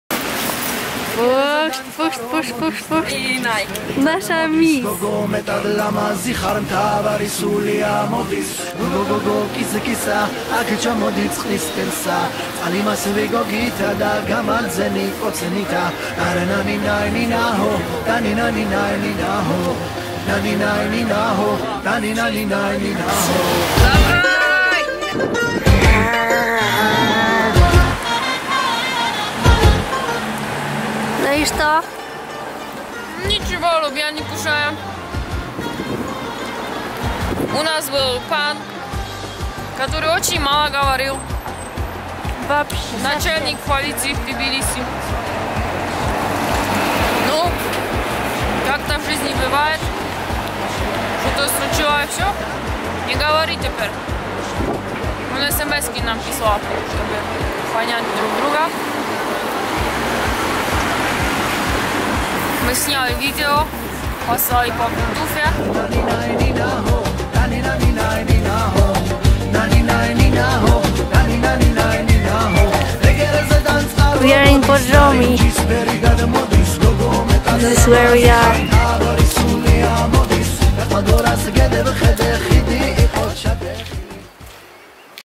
Arbuzi Way to Borjomi
The journey starts in Batumi and our hopuse 50 m from the Black Sea. You can notice freestyle on Georgian roads and some landscapes. The journey ends in Tusheti and our hostel in Omalo. Feel invited!